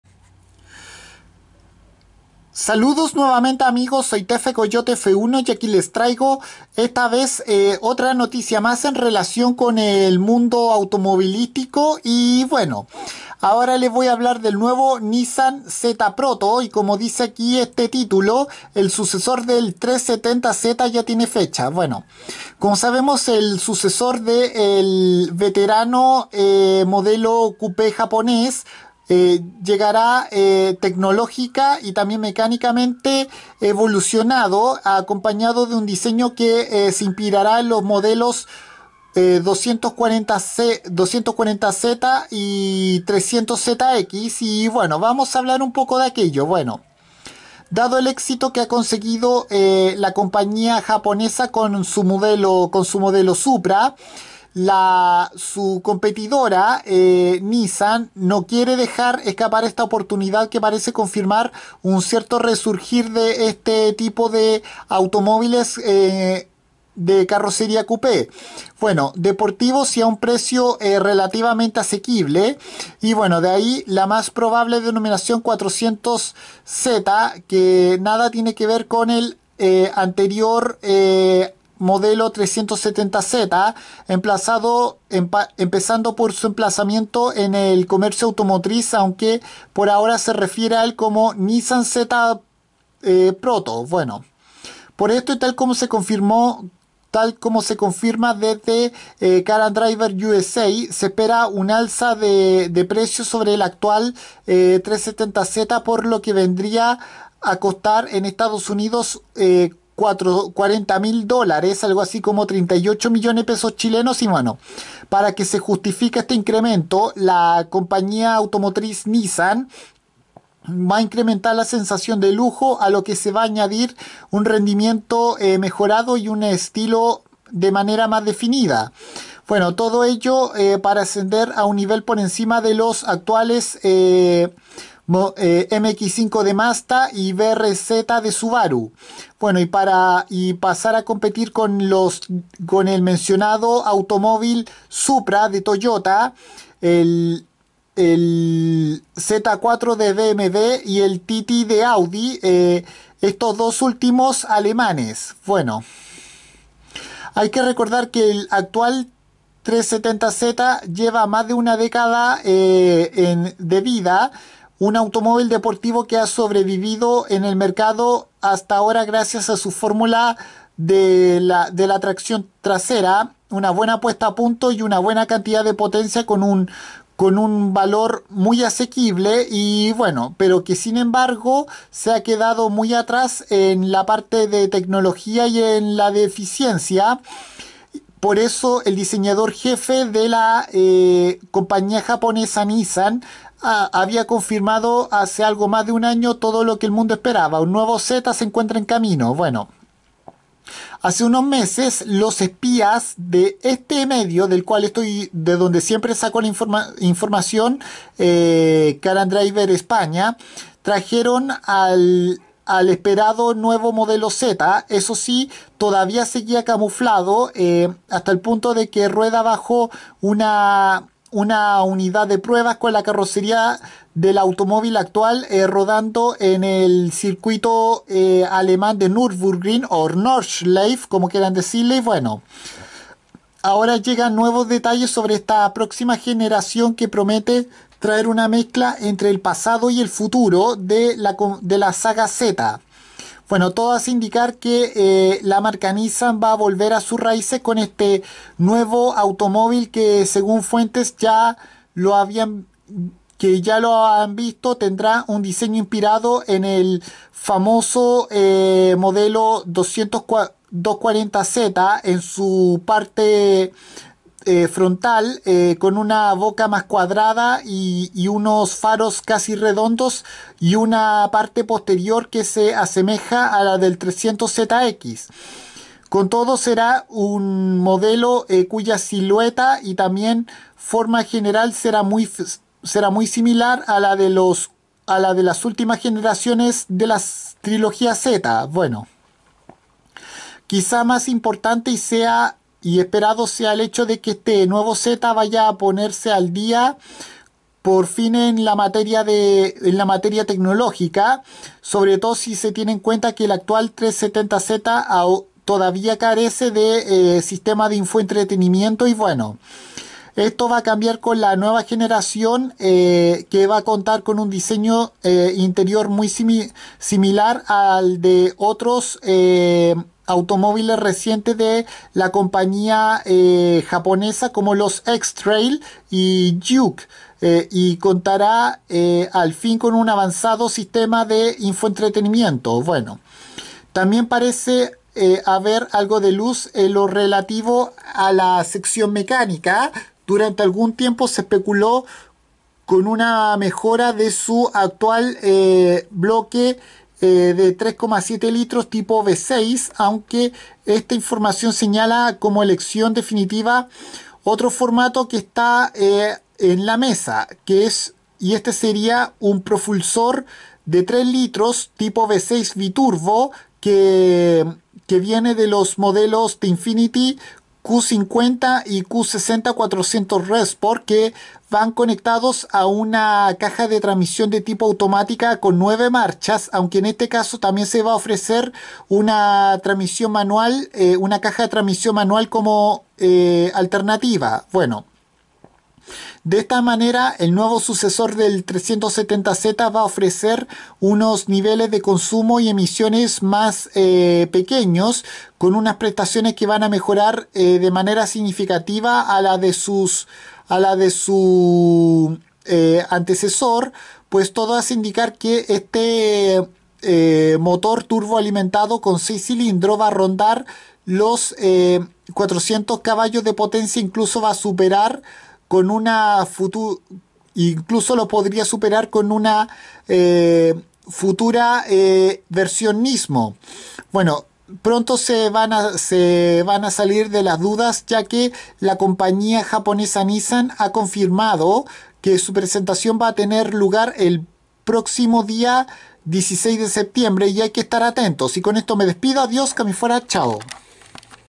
Nissan Z Proto información
Será el sucesor del 370Z y este nuevo automóvil deportivo barato de la compañía automotriz japonesa Nissan tendrá nueva evoluciones tanto en mecánica como en tecnología y con diseños inspirados en los clásicos modelos 300ZX y 240Z